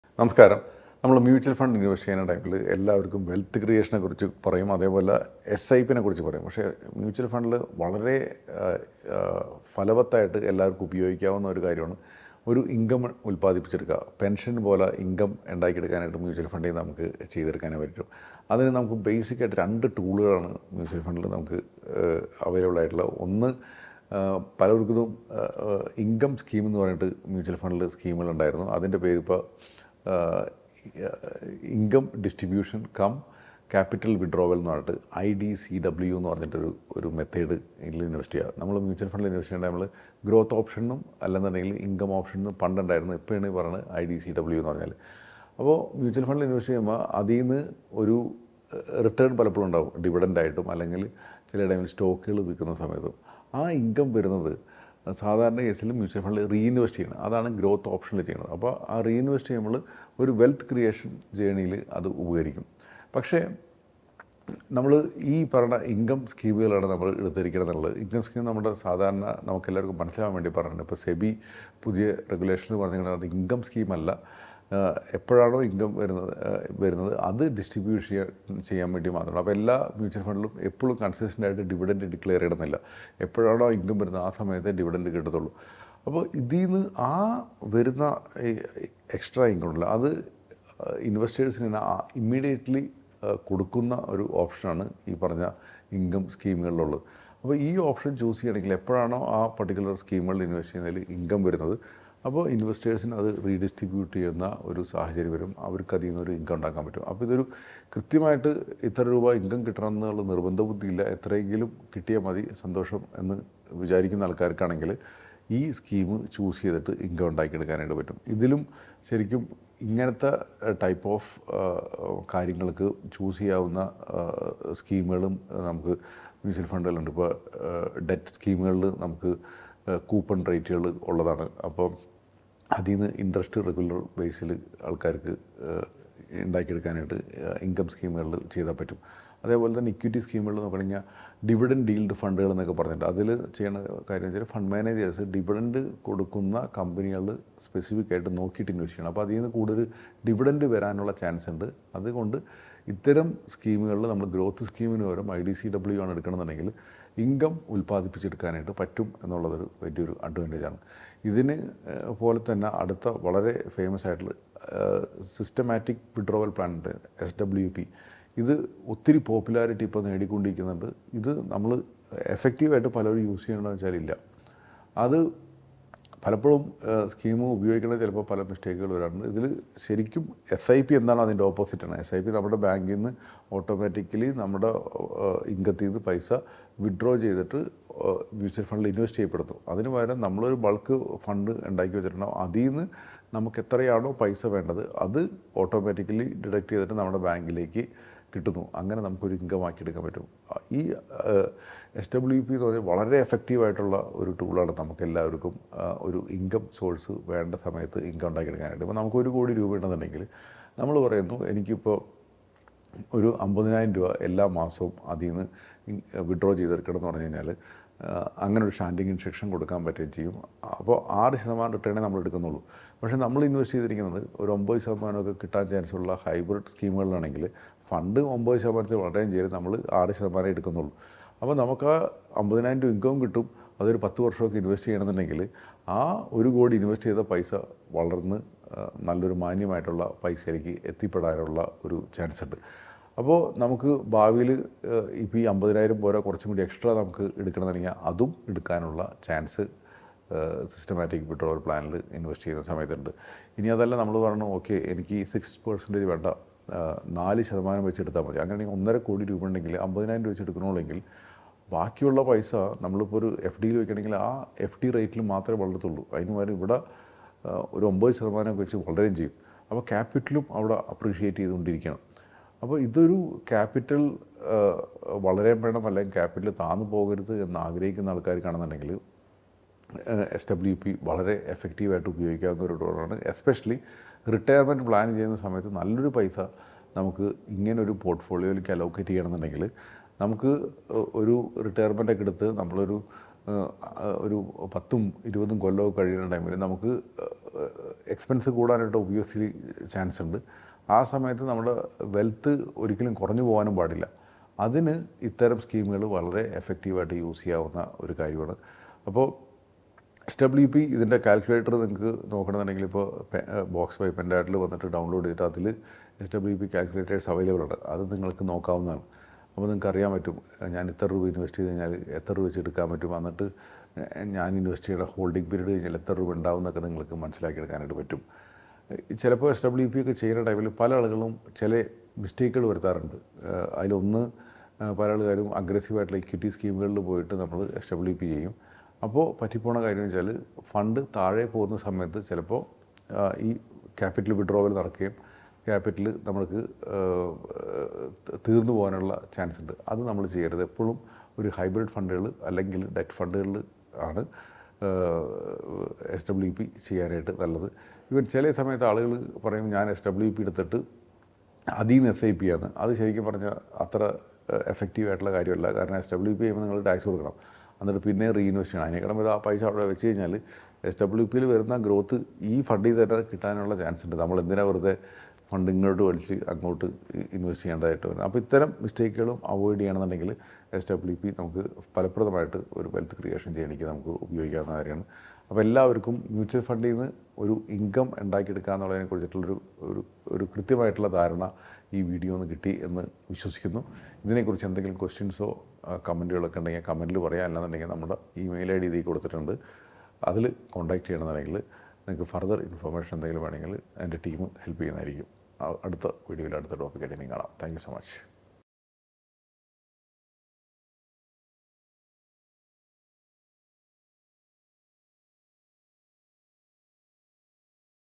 How to Earn While Investing in Mutual Funds | Earn Income from Mutual Funds
Tired of just watching your mutual fund investments grow? Learn two powerful strategies to generate income while still enjoying the benefits of long-term growth. Discover how to maximize your returns and achieve financial freedom faster.